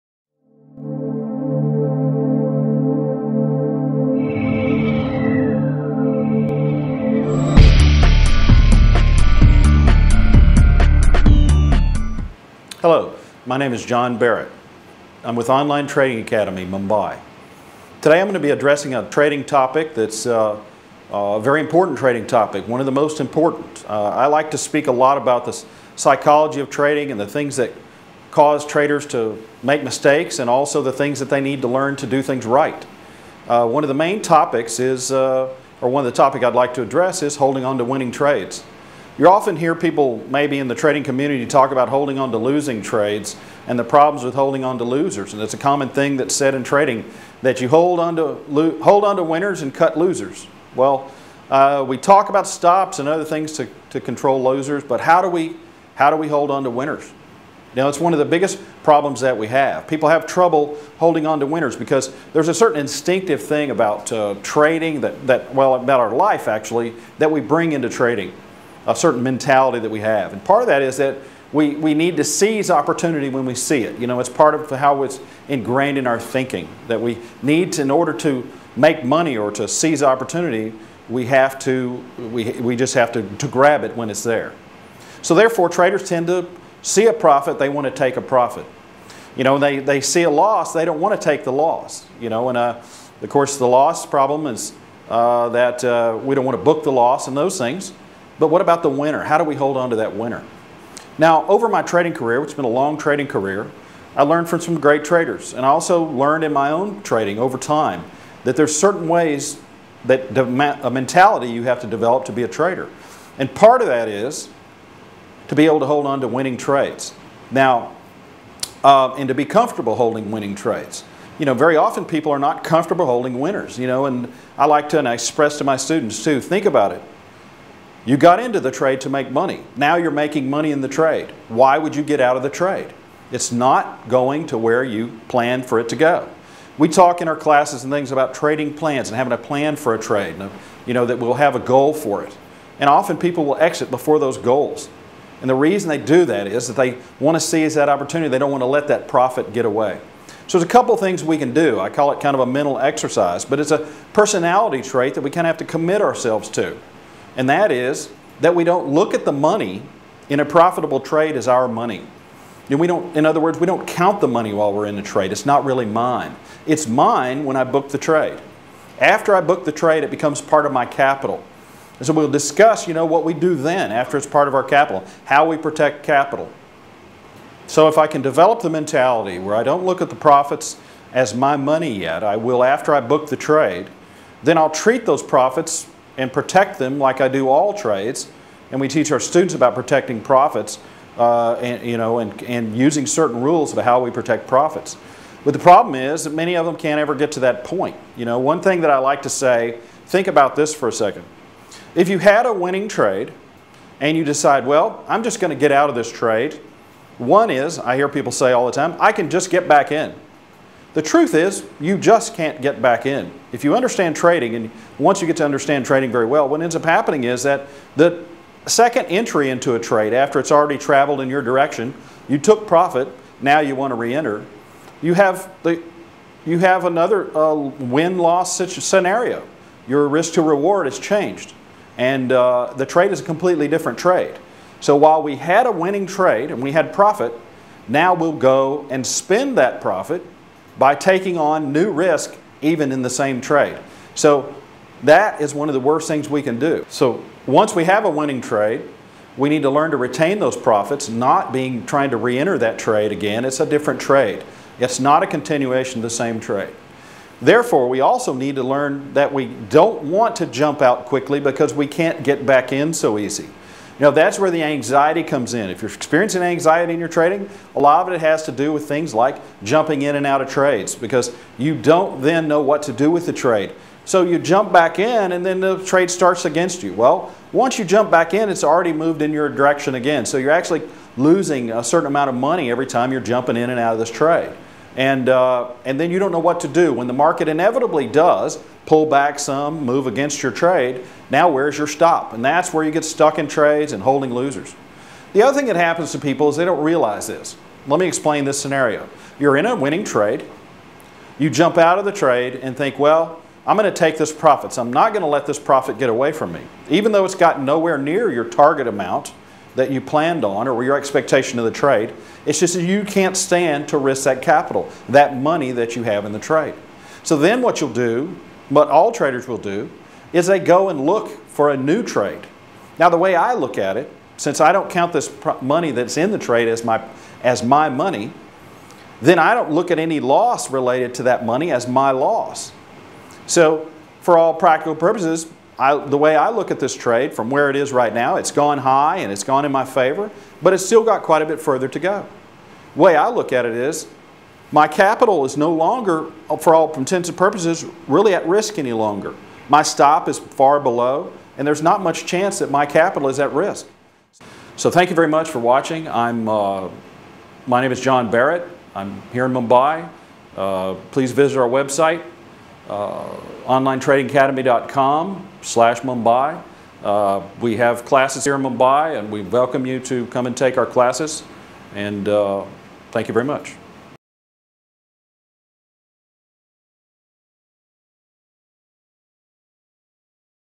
How to hold onto 'Winning Trades'?
Online Trading Academy's John Barrett explains how to hold onto 'Winning Trades'